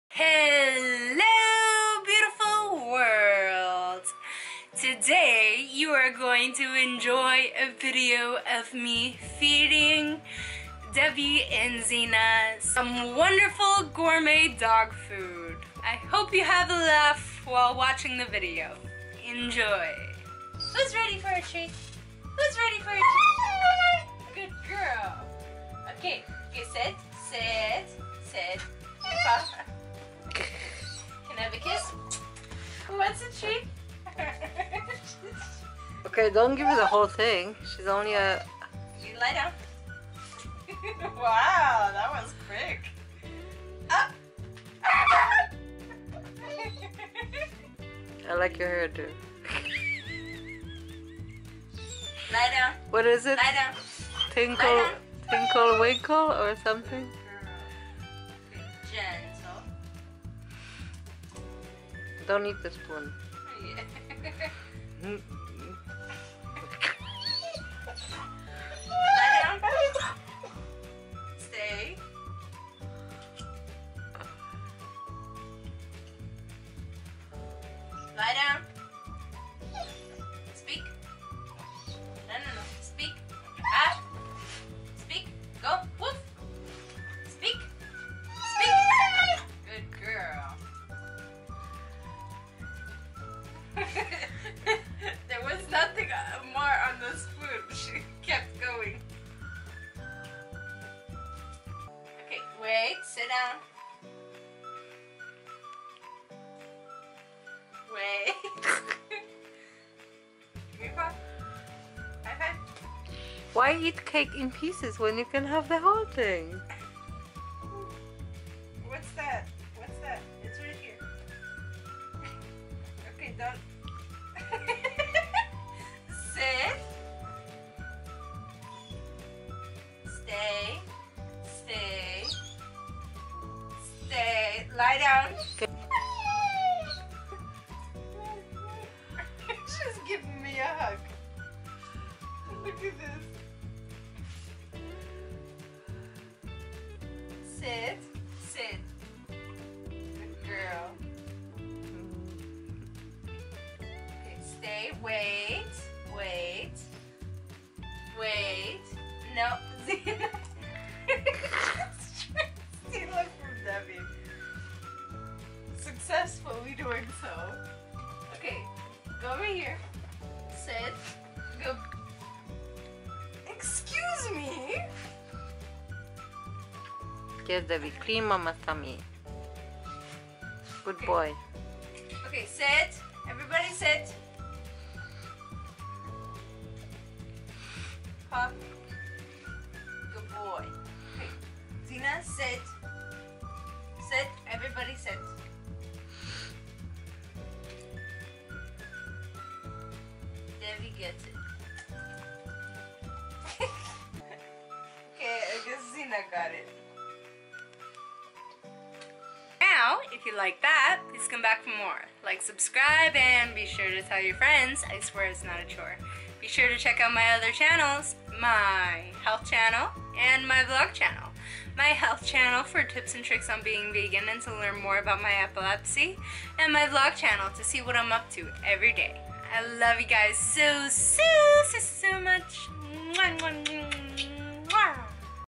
Funny Fancy Dogs Eat Gourmet
It's funny to see how dogs react when they're about to get their version of junk food.